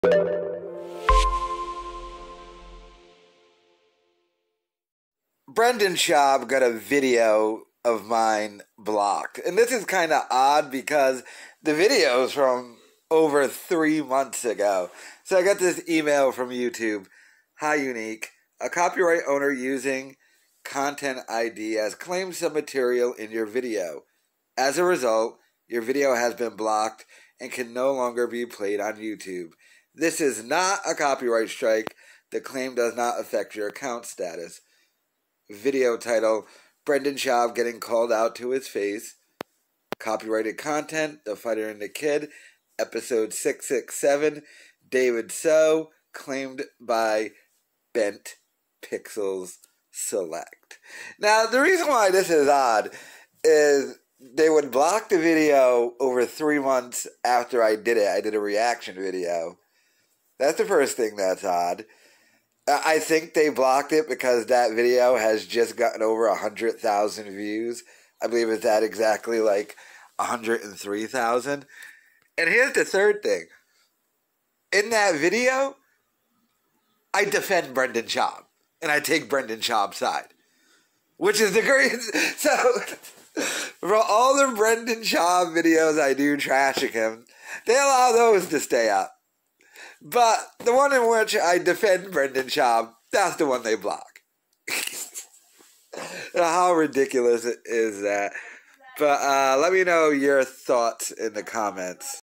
Brendan Schaub BLOCKED My Video About Him!!!
the fighter and the kid host blocked my youtube video about